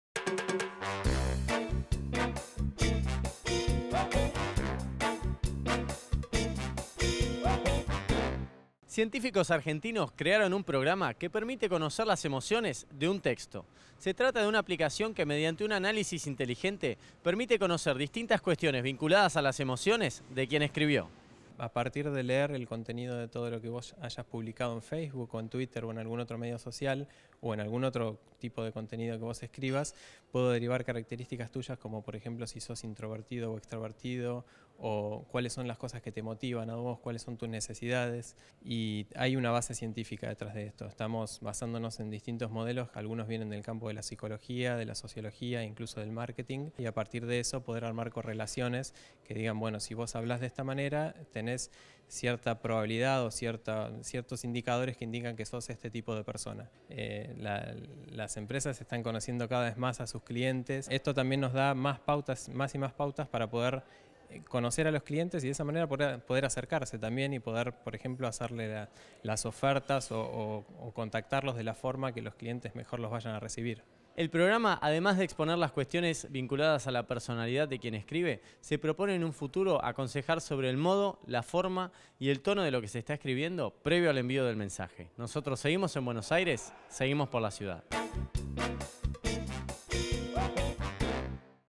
"Personality Insights", la nueva app que lee emociones en los textos - Por la ciudad
Científicos argentinos crearon un programa que permite conocer las emociones de un texto. Se trata de "Personality Insights", una aplicación que mediante un análisis inteligente permite conocer distintas cuestiones vinculadas a las emociones de quien escribió.